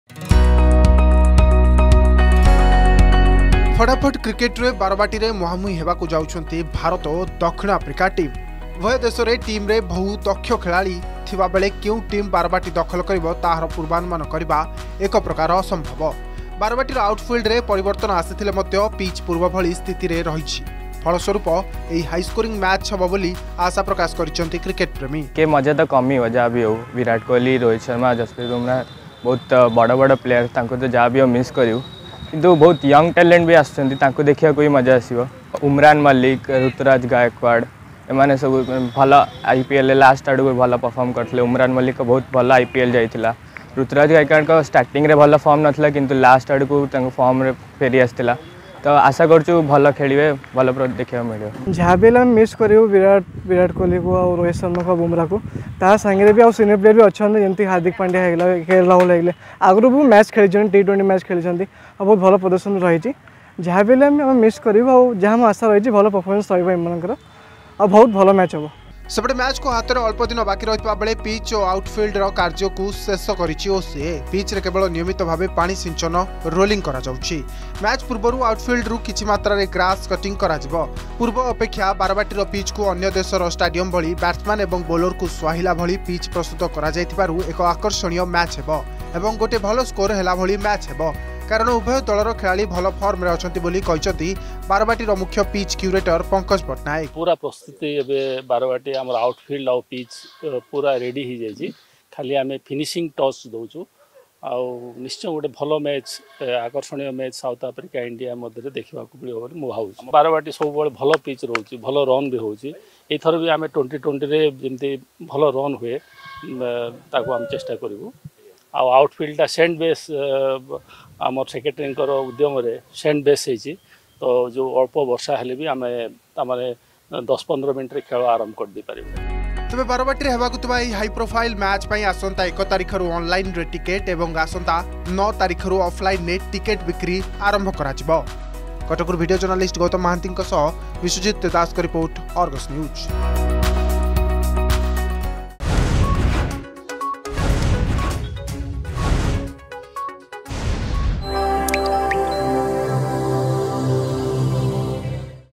ଫଟାଫଟ କ୍ରିକେଟ ରେ ବାରବାଟୀରେ ମୁହାଁମୁହିଁ ହେବାକୁ ଯାଉଛନ୍ତି ଭାରତ ଓ ଦକ୍ଷିଣ ଆଫ୍ରିକା ଟିମ
ବାରବାଟୀ ଟି-20 ମ୍ୟାଚକୁ ଆଉ ହାତରେ ମାତ୍ର ଦିନ କେଇଟା ରହିଛି ସେଥିପାଇଁ ପିଚ ଠାରୁ ଆରମ୍ଭ କରି ଗ୍ୟାଲେରୀ ପ୍ରସ୍ତୁତି ଶେଷ ପର୍ଯ୍ୟାୟରେ ପହଞ୍ଚିଛି । ଦିର୍ଘ ଅଢେଇ ବର୍ଷ ପରେ ନିଜର ପସନ୍ଦର ଖୋଳାଳିମାନଙ୍କୁ ବାରବାଟୀରେ ଖେଳିବାର ଦୃଶ୍ୟକୁ ଉପଭୋଗ କରିବା ପାଇଁ ଉତ୍ସହର ସହ ଅପେକ୍ଷା କରିଛନ୍ତି ଦର୍ଶକ ।